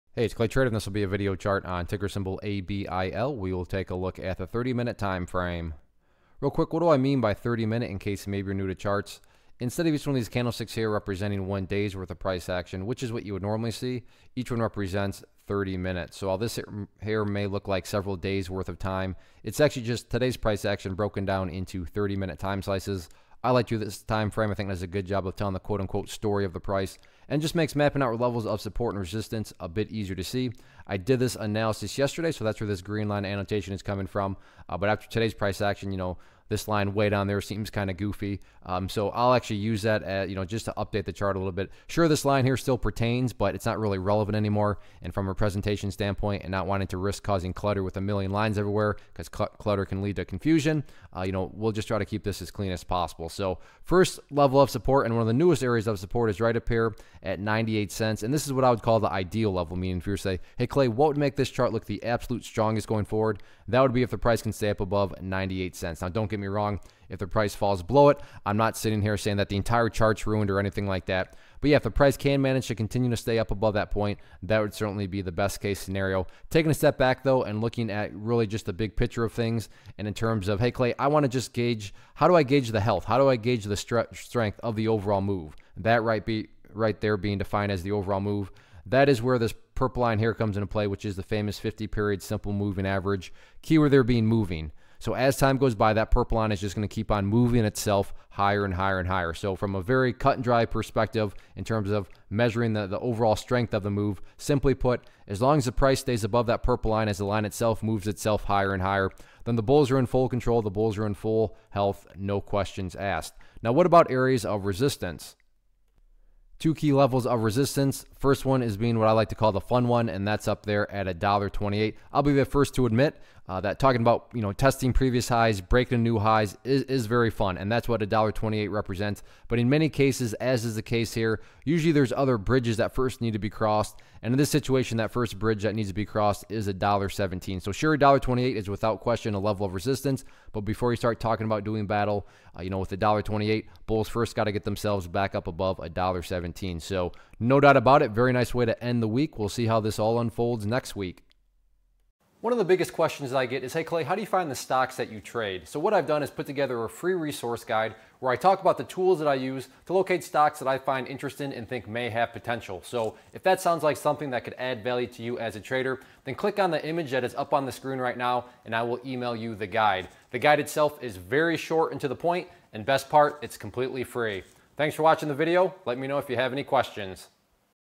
Ability Inc - ABIL Stock Chart Technical Analysis for 08-25-17
Learn how to read stock charts and identify technical patterns as ClayTrader does a quick stock chart review on Ability Inc (ABIL).